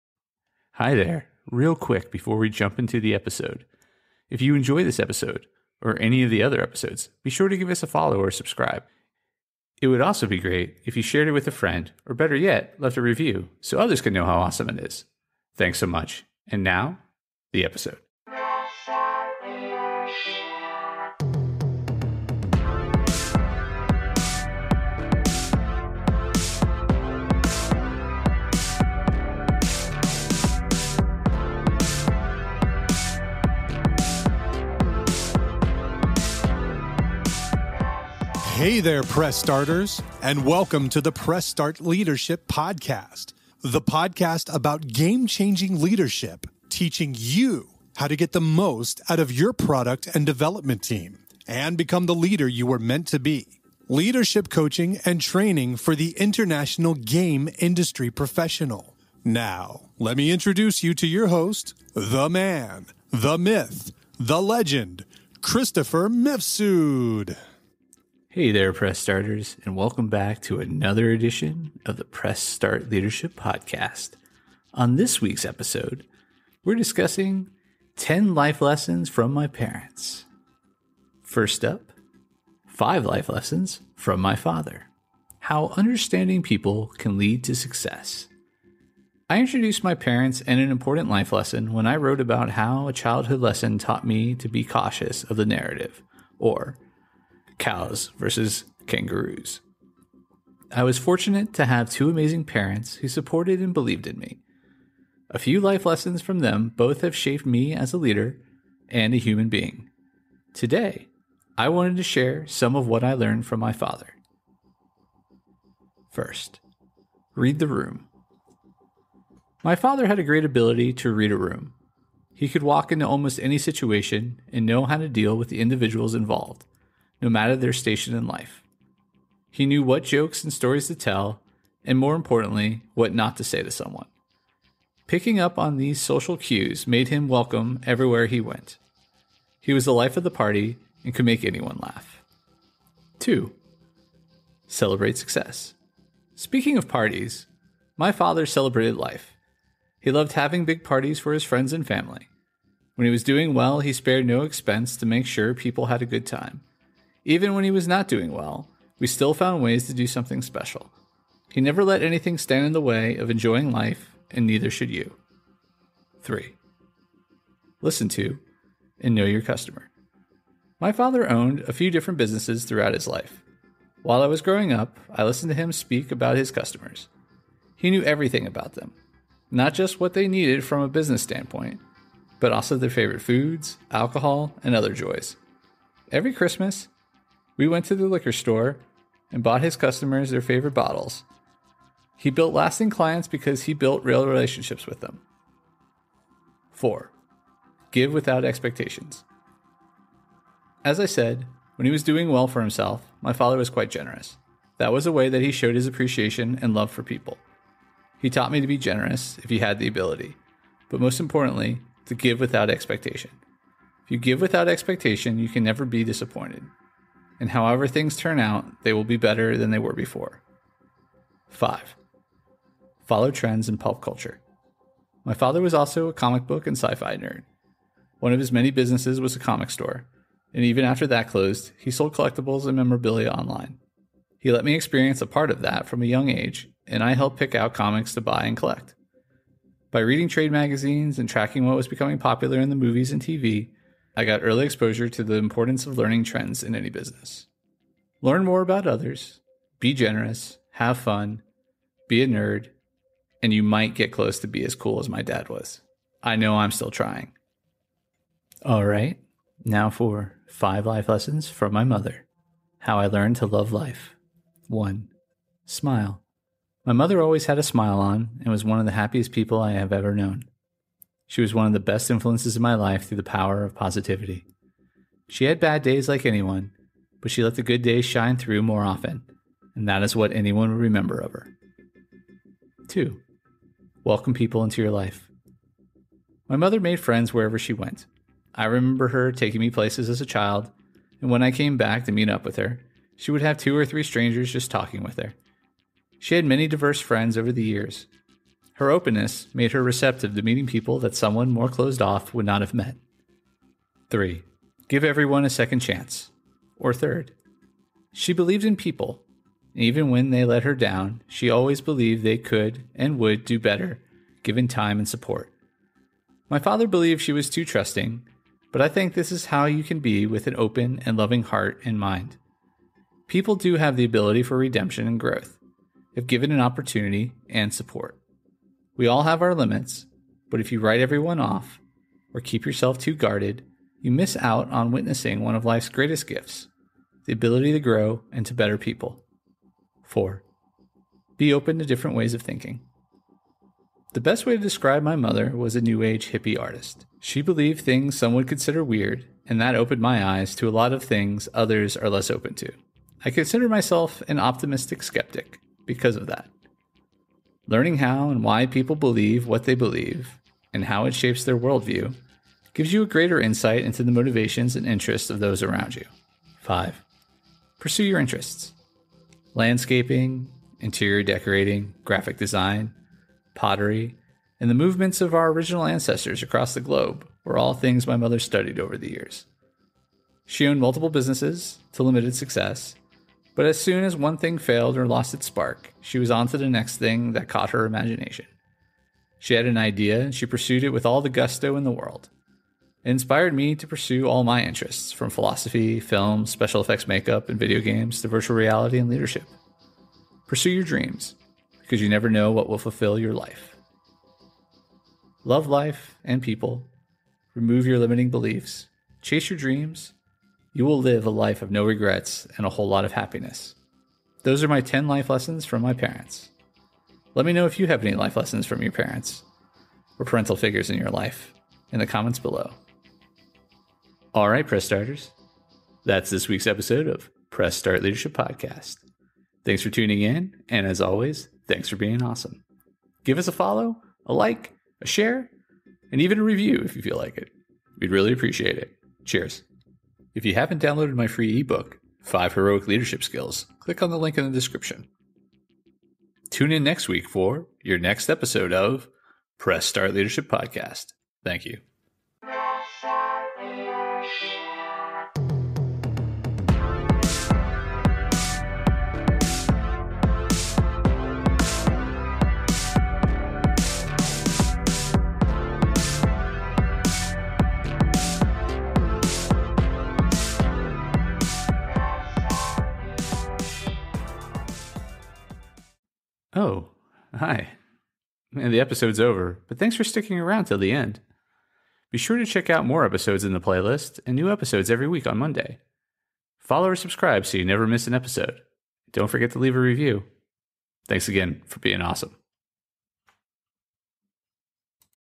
10 Life Lessons From My Parents
On this week's episode of Press Start Leadership Podcast, we discuss:

5 Life Lessons from my Father

5 Life Lessons from my Mother

Intro and background music by: Joey the Mad Scientist